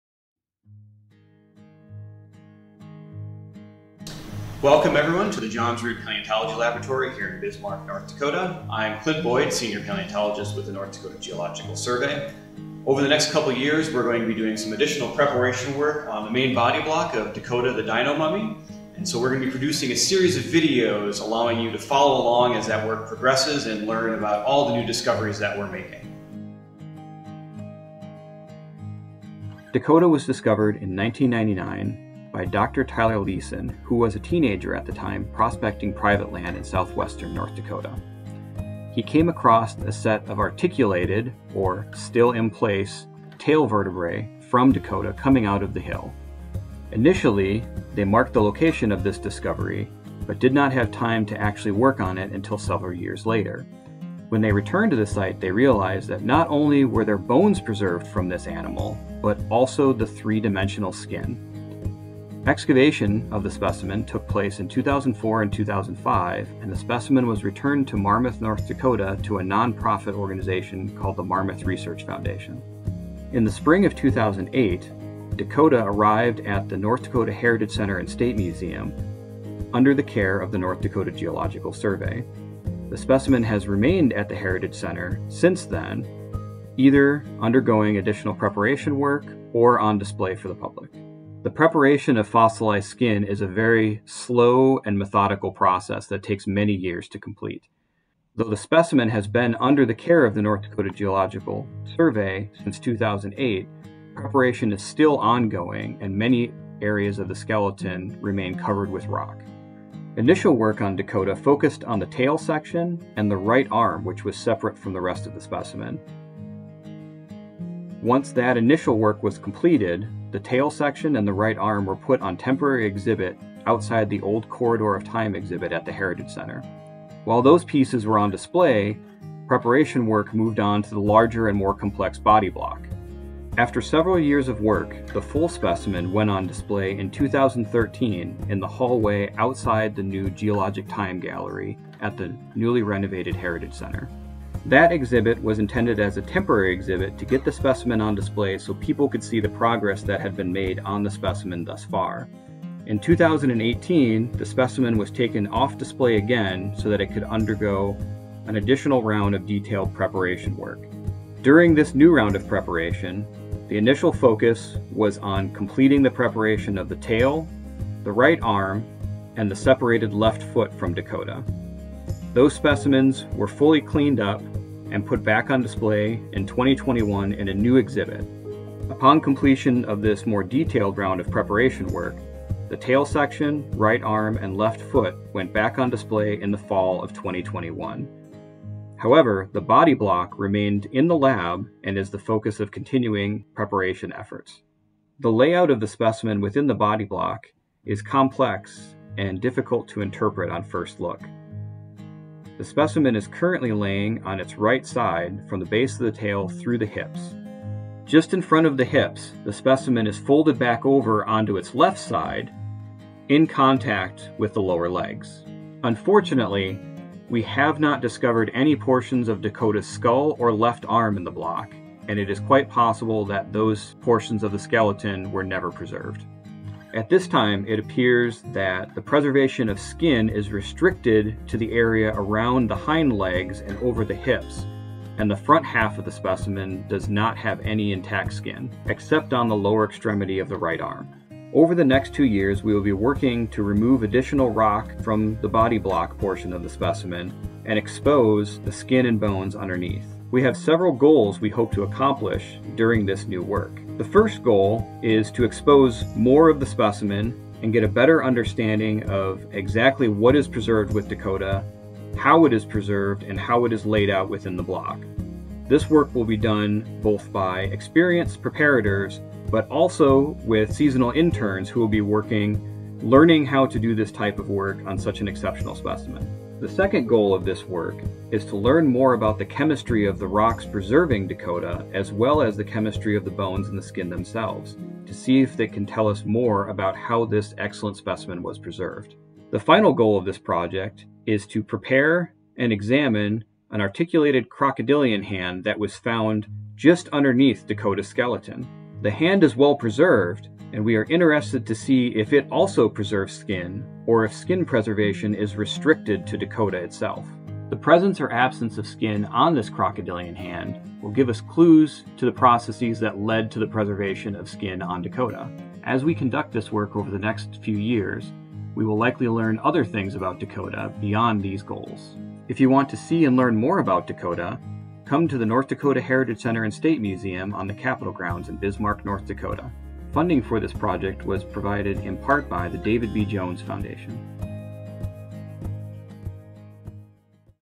Deciphering the Dinomummy: Introduction
Join the North Dakota Geological Survey Paleontology department as they continue unlocking the mysteries surrounding "Dakota the Dinomummy" - an exceptionally preserved Edmontosaurus dinosaur with fossilized skin.

For updates throughout the year, please find us @ndgspaleo on social media (Facebook, Instagram, Twitter, Mastodon)

Intro illustration by Natee Himmapaan (Puttapipat)